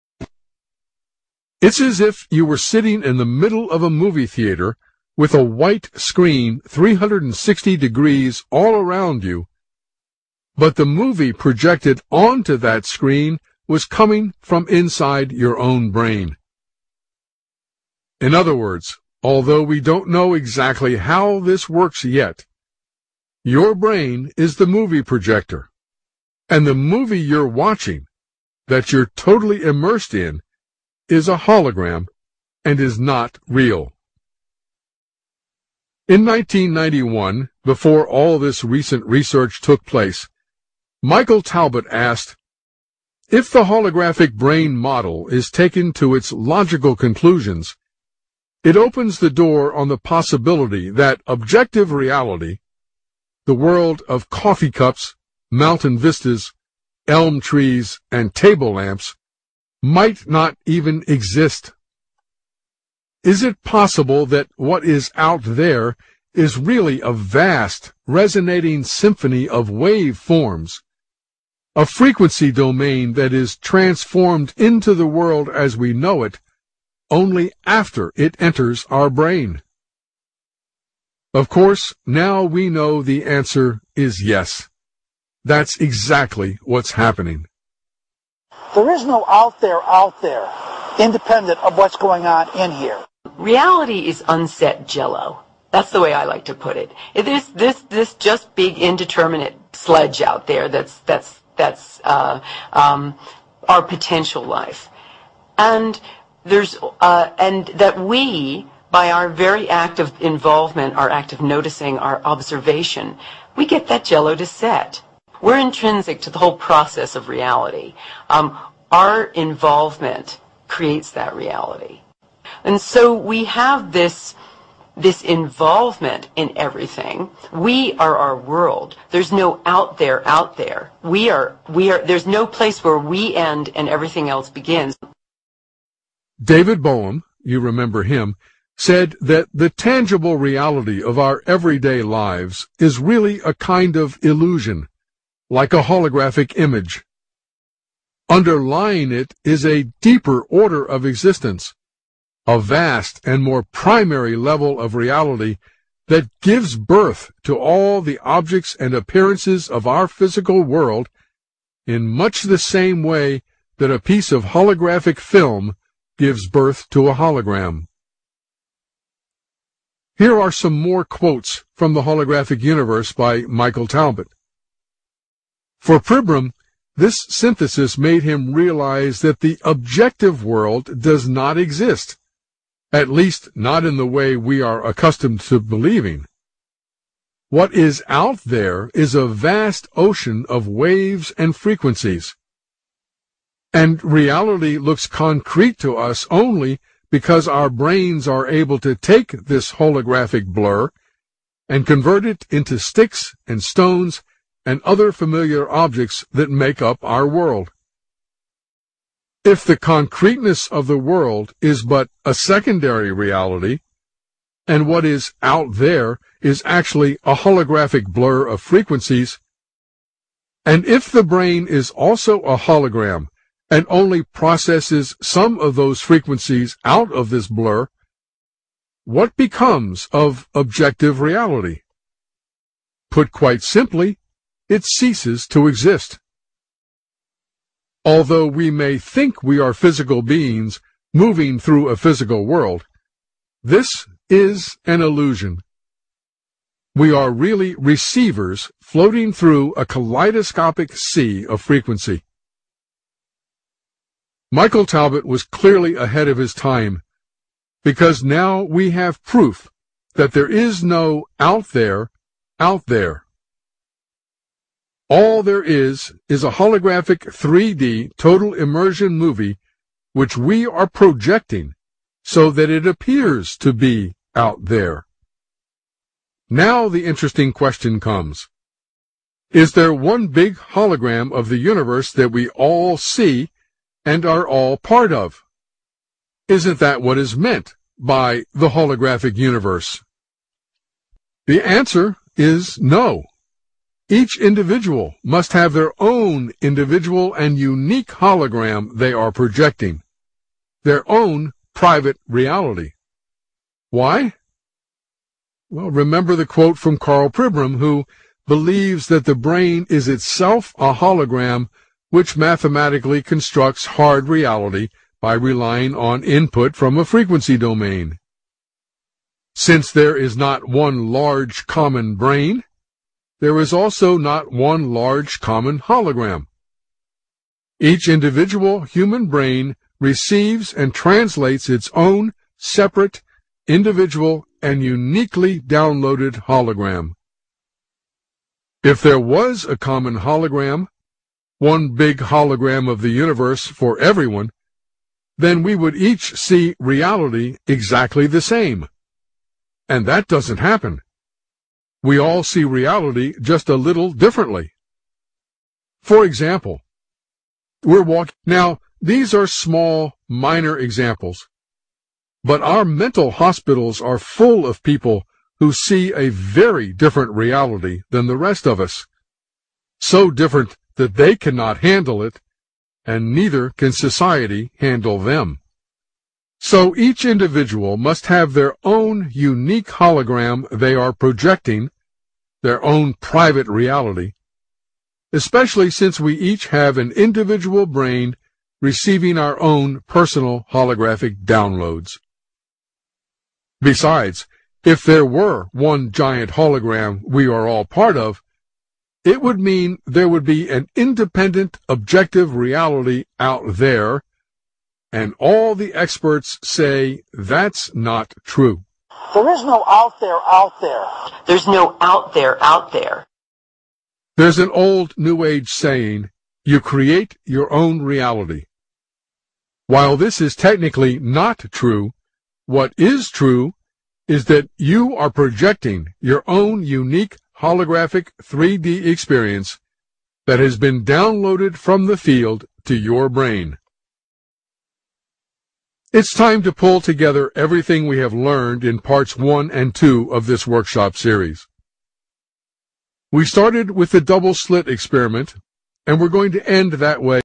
Universe 7 - Mỗi Người Có Vũ Trụ của Riêng Mình - Phụ đề Việt ngữ
Khoa học hiện nay xác nhận rằng Không Có một vũ trụ độc lập khách quan, chỉ có vũ trụ chủ quan riêng của mỗi người tải xuống từ Trường tần số vô hình đến bộ não của họ và biến đổi thành cái gọi là "thực tại khách quan". Sự vật bên ngoài chỉ là ảo tưởng. Đó cũng chính là cái Phật giáo nói thế gian là huyễn ảo, đó chỉ là cộng nghiệp và biệt nghiệp của chúng sinh tạo ra.